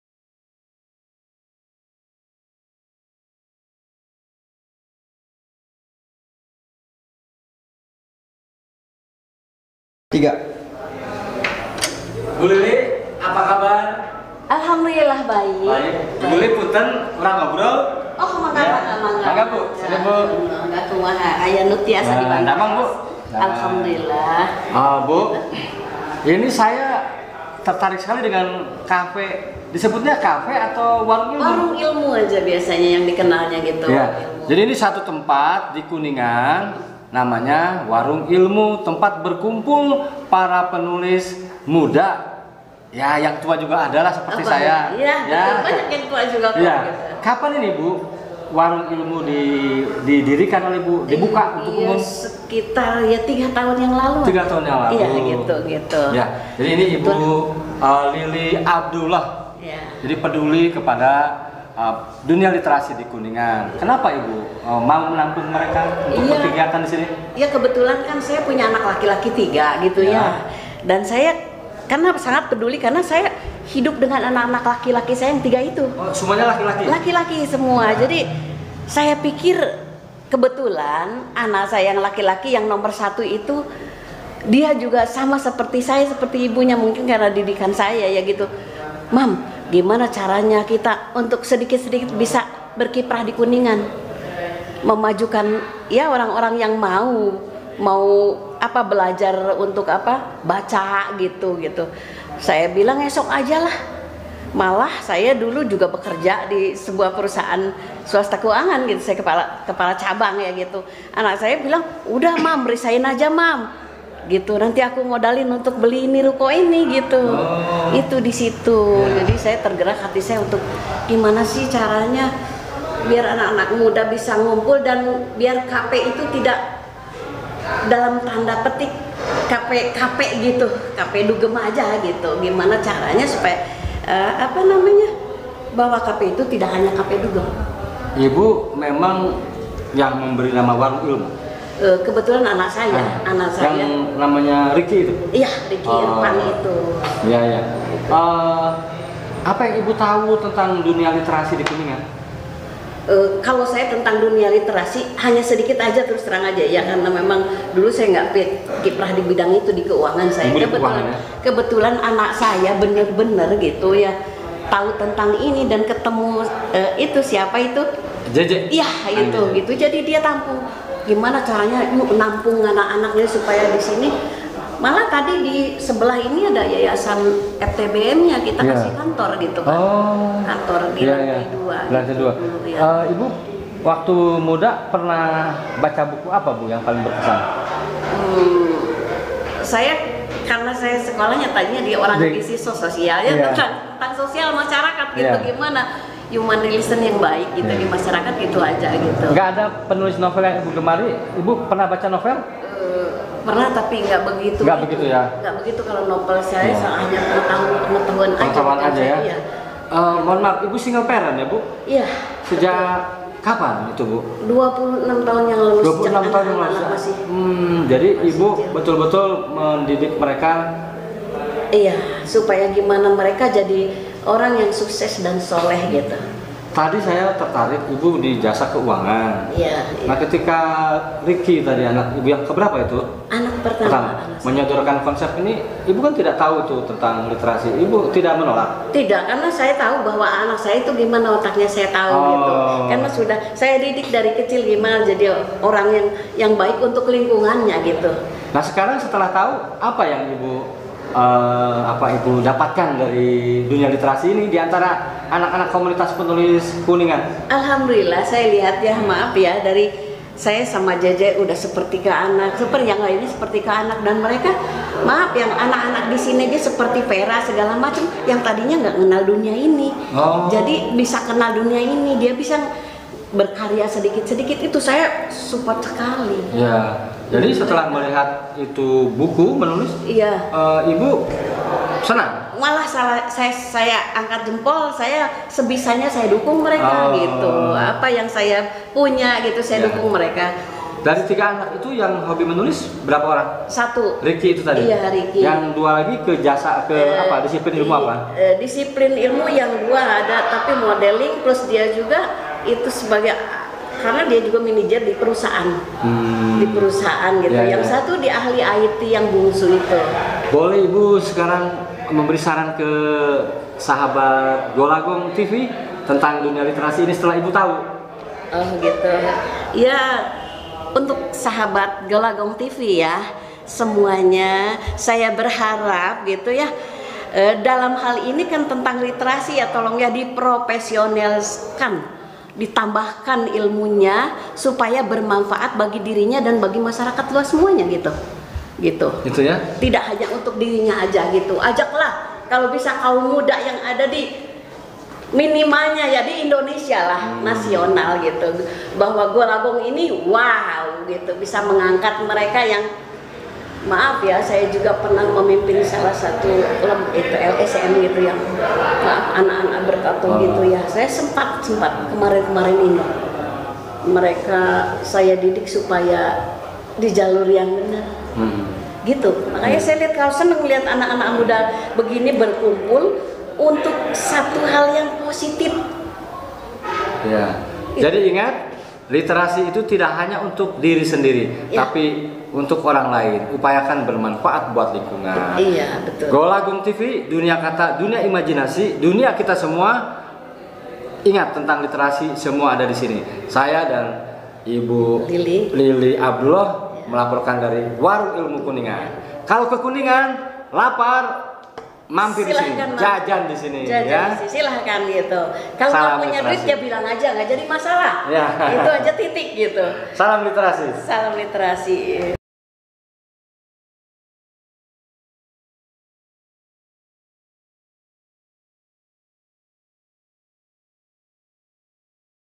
Warung Ilmu Peduli Literasi Kuningan
Komunitas Penulis Muda Kuningan atau Forum TBM Kuningan sering menggunakan Warung Ilmu untuk kegiatan literasinya.